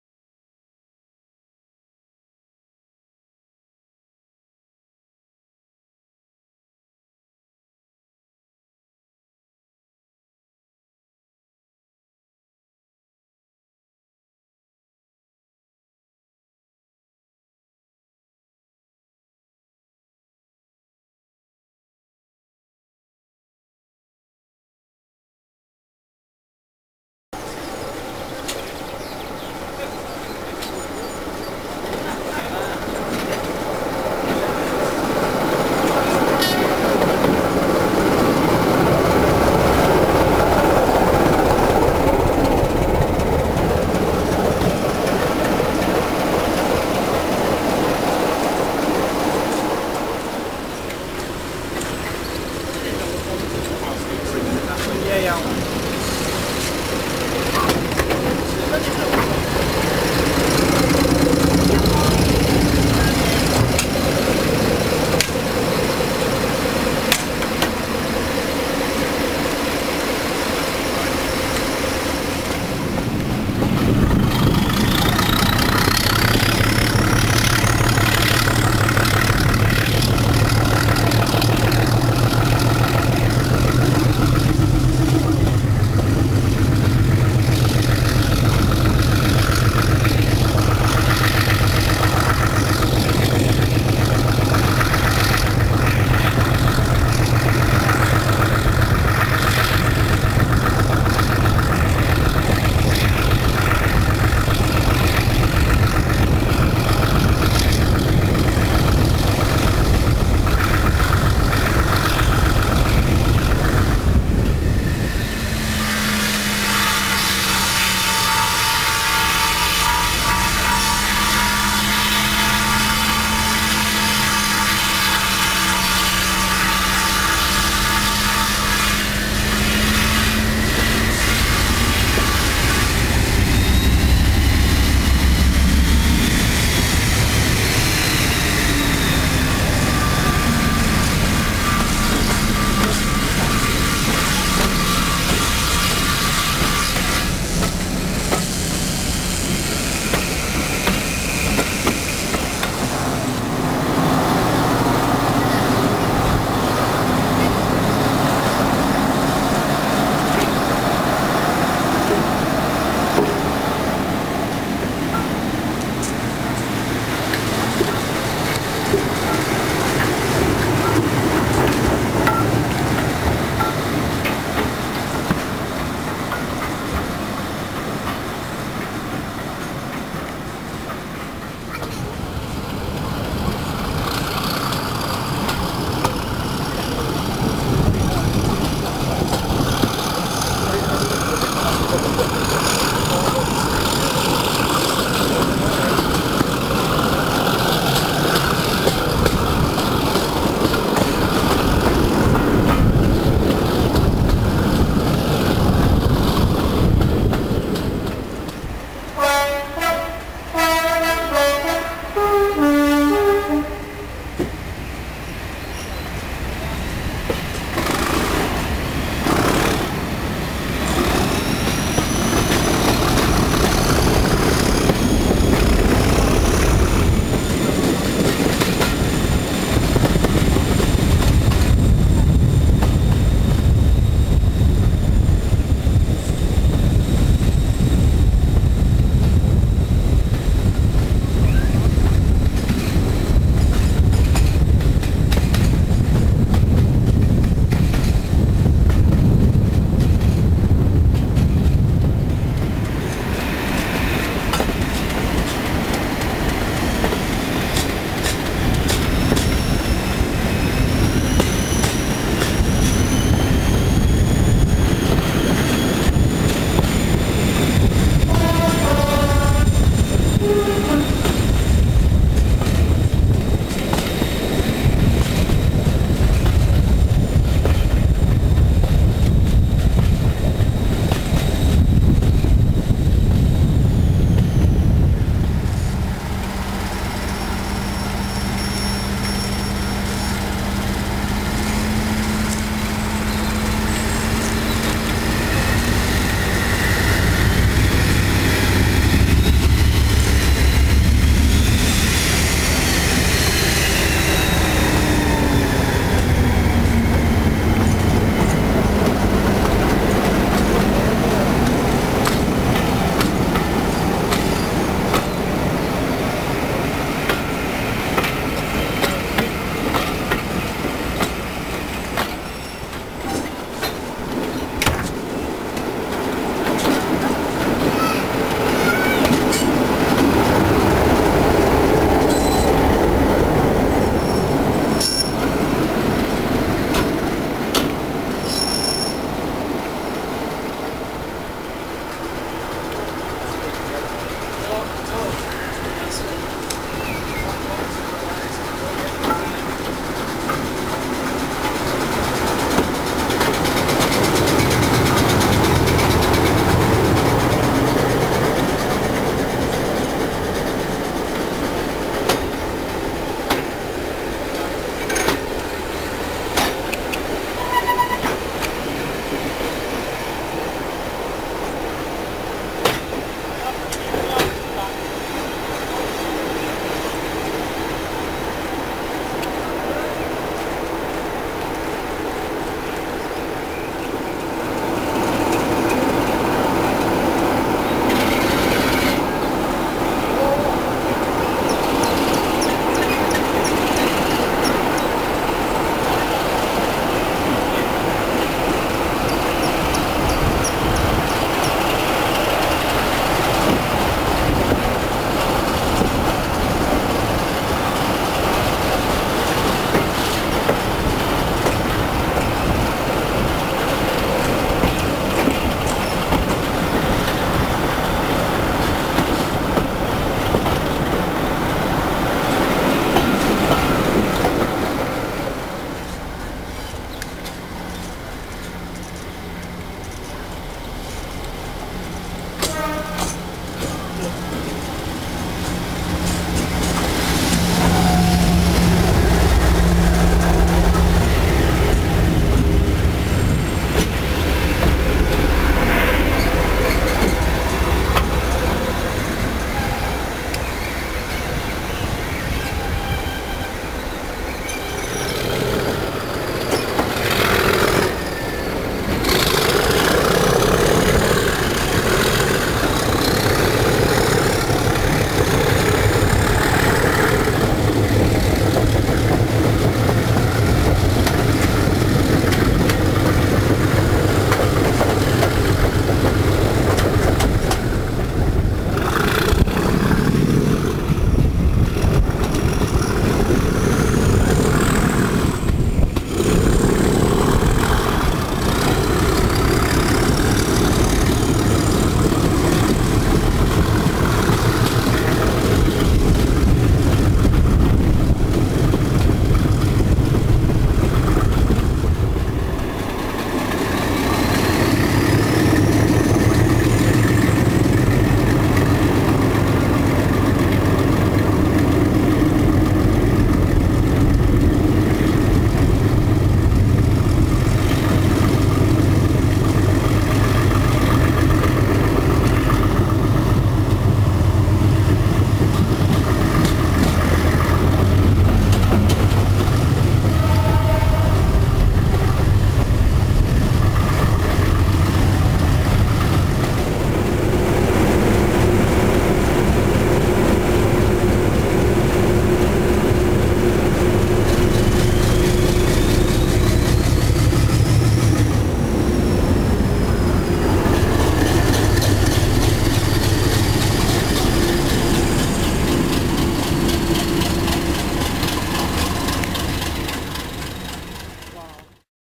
Battlefield Line Spring Diesel Gala, 25/04/09
A visit to the Battlefield Line in Leicestershire for their 2009 Spring Diesel, featuring an impressive diesel line-up. The line-up featured class 02 No. D2867, class 03 No. 03170, class 04 No. 04110, class 25 No. 25067, class 31s Nos. 31101 and 31130, class 33 No. 33019, class 37s Nos. 37227 and 37905, class 47 No. 47640, class 56 No. 56098 and class 73 No. 73114. This video features most of these diesels in action.

Enjoy the thrash!